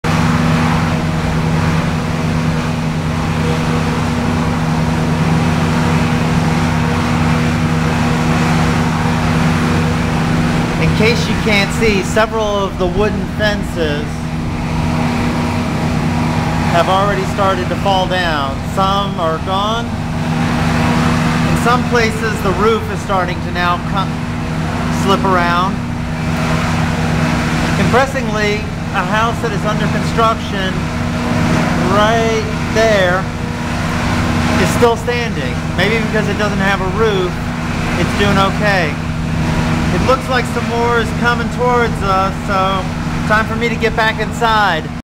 Damaged Fences
Fences across the street from the hospital falling down from Gustav